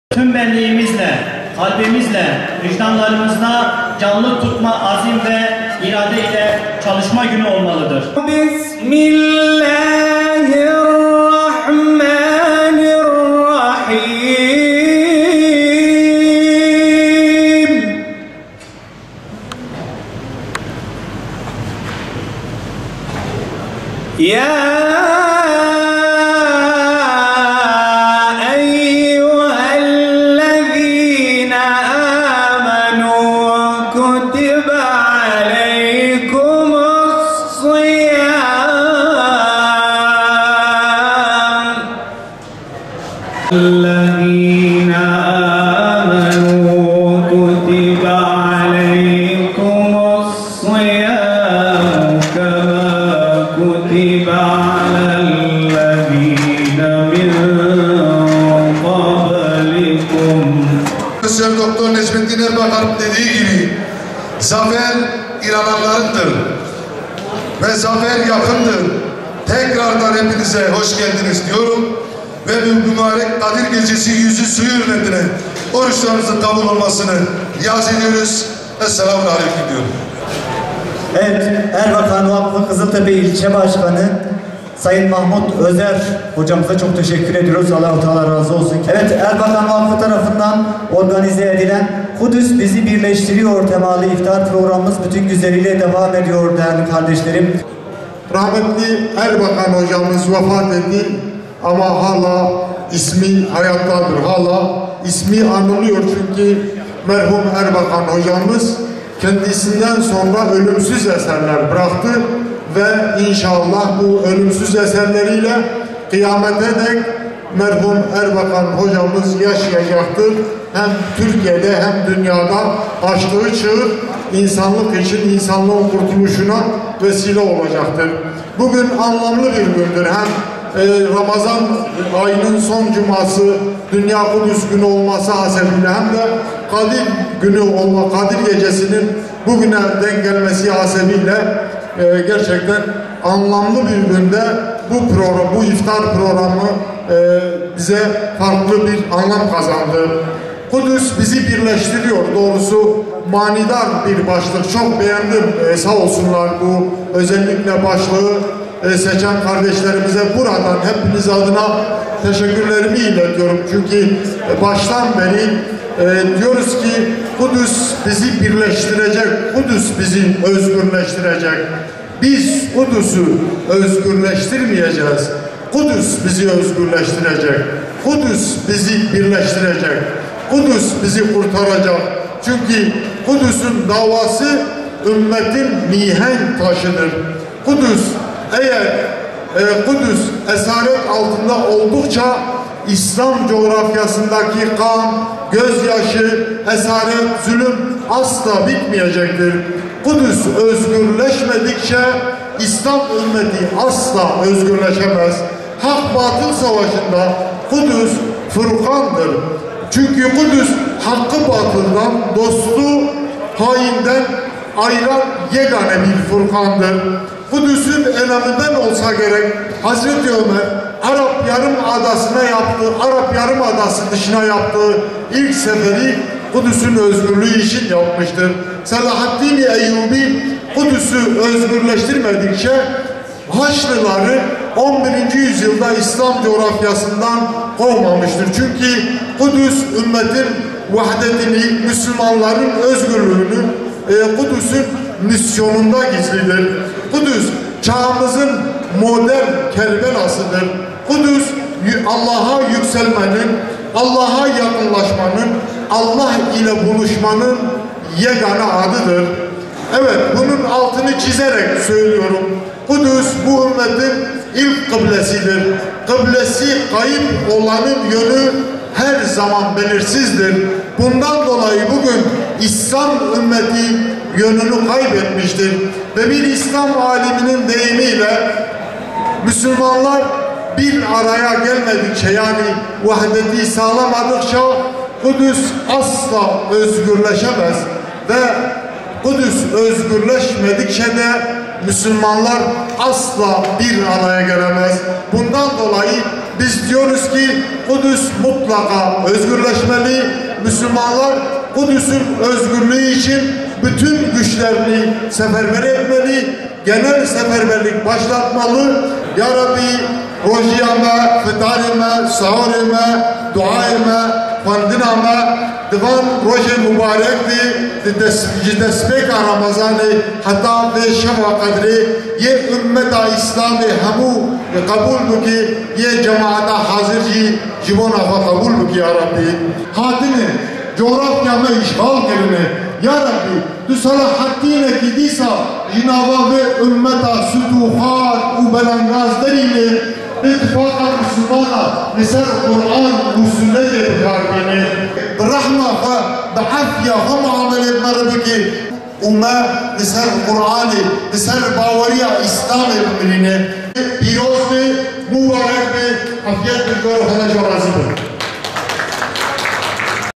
"Kudüs özgürleşmedikçe İslam ümmeti özgürleşemez"
HÜDA PAR Mardin Milletvekili adayı Kılınç, Kudüs özgürleşmedikçe İslam ümmetinin asla özgürleşmeyeceğini belirtti.